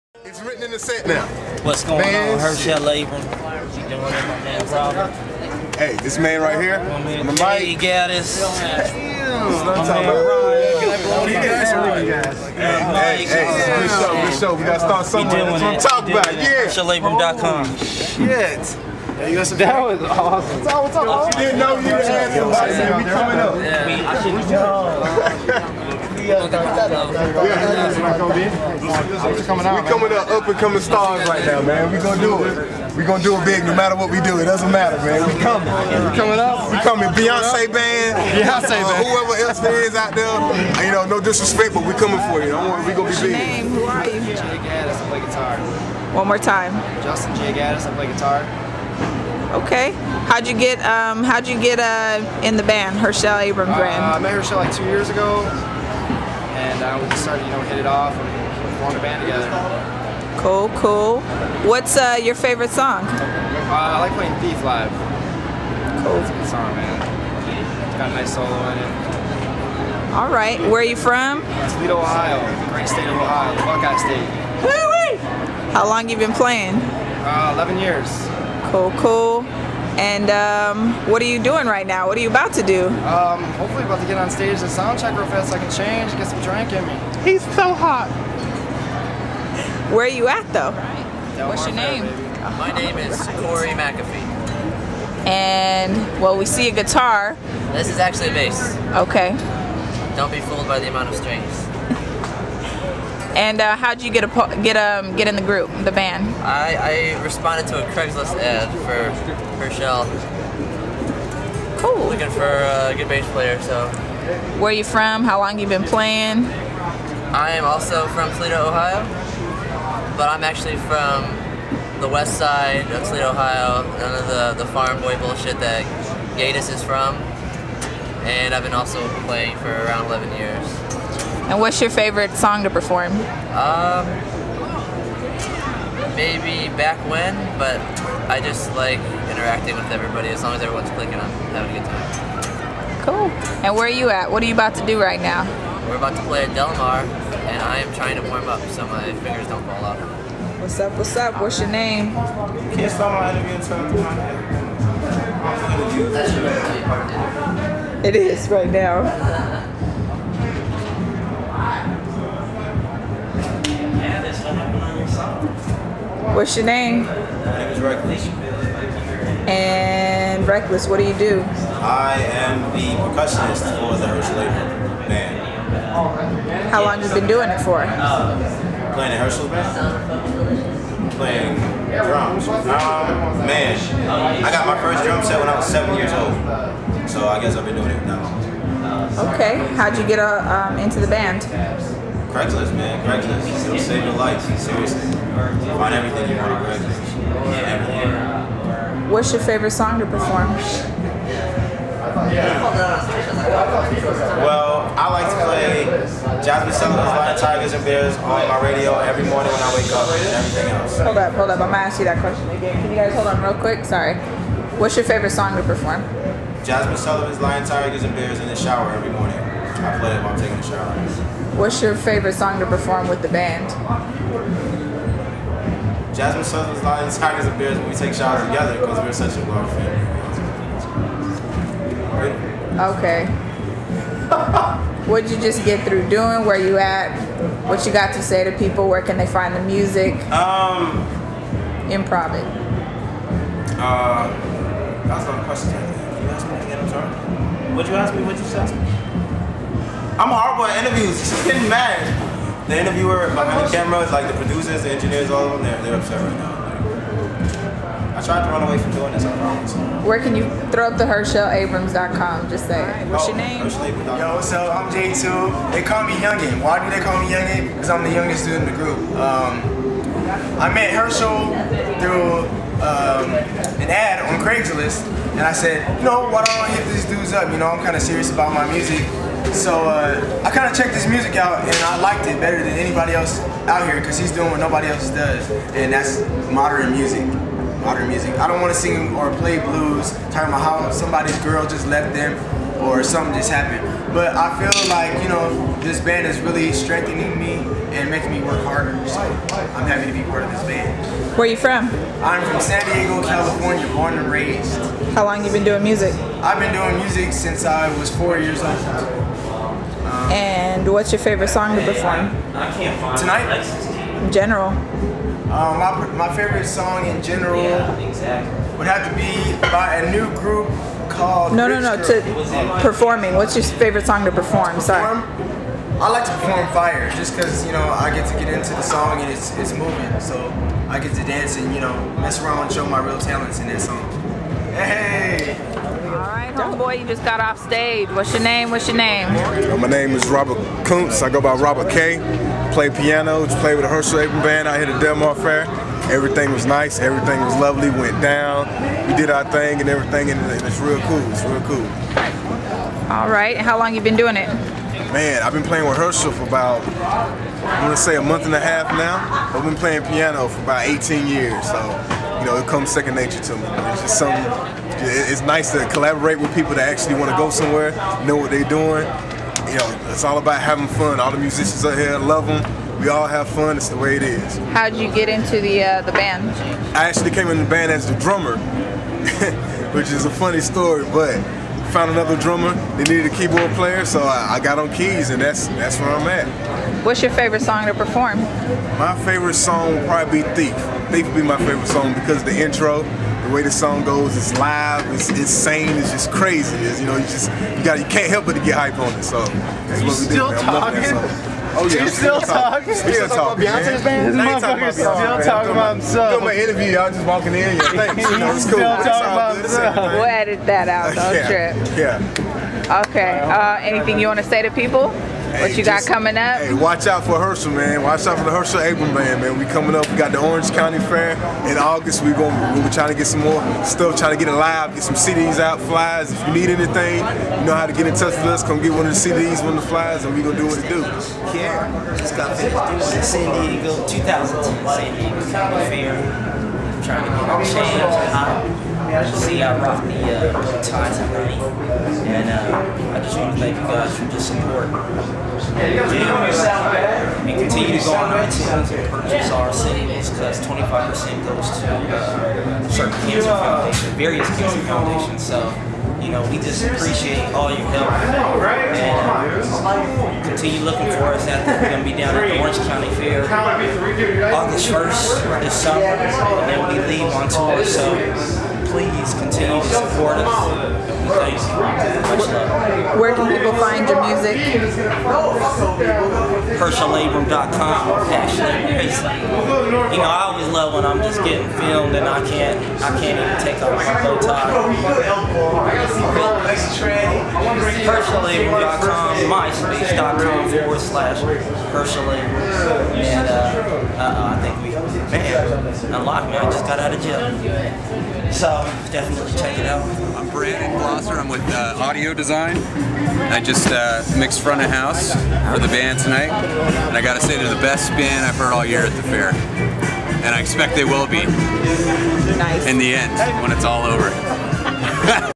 Hershel Abram | San Diego County Fair | Interviews
BACK STAGE INTERVIEWS OF THE HERSHEL ABRAM BAND